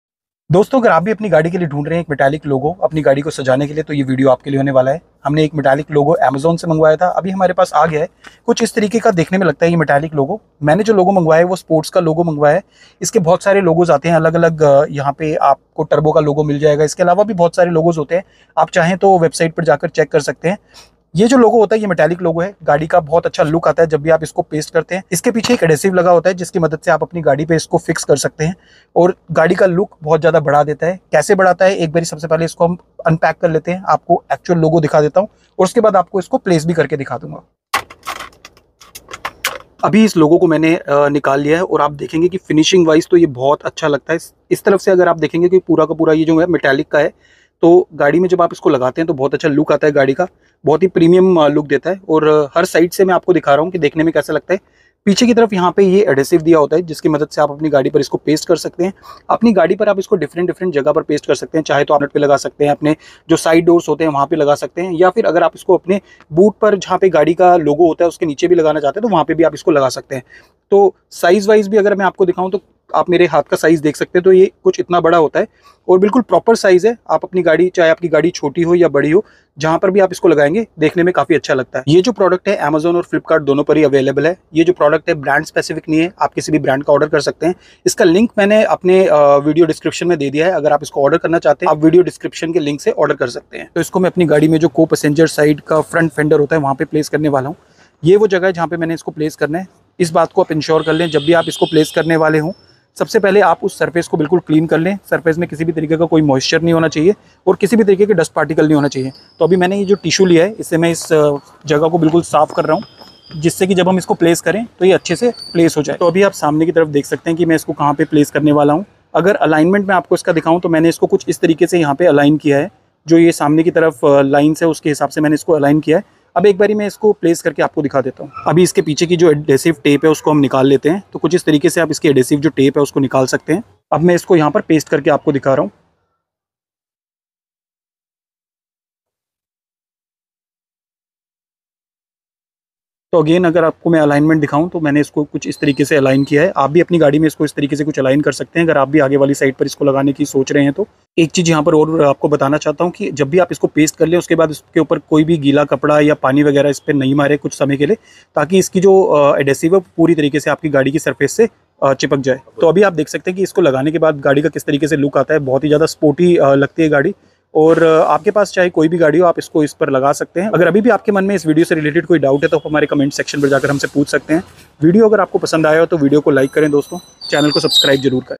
Sports Logo Metal Sticker Installation | MG Hector Modification | Car Sticker Installation
Welcome to our comprehensive Installation of Sports Metal Chrome Logo In MG Hector.In this video, we dive deep into the features, performance, and overall quality of this highly anticipated product.

Whether you're considering purchasing it or simply curious about its capabilities, this review will provide you with all the essential information you need.